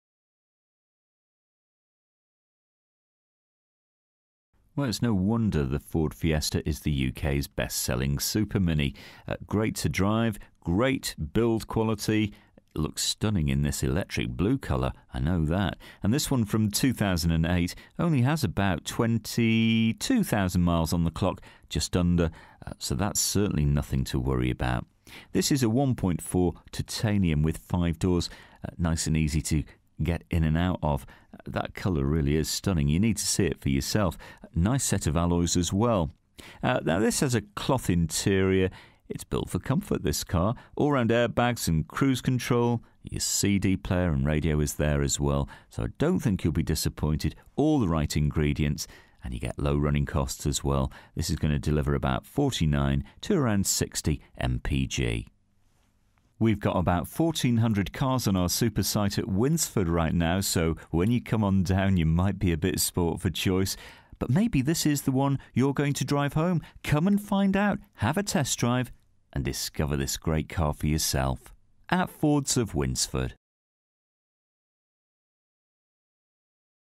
FORD FIESTA HATCHBACK (2008) 1.4 TITANIUM 5DR - YP58KTG
DETAILS:
First registered: 21/11/2008
Current mileage: 21720
Registration: YP58KTG
Colour: BLUE

FEATURES:
front electric windows, remote central locking, cloth interior, all round airbags, climate control, pas, abs, alloy wheels, metallic paint, cd, parcel shelf present, hand book present, cruise contro

This used car is for sale at Fords of Winsford Car Supermarket in Cheshire, UK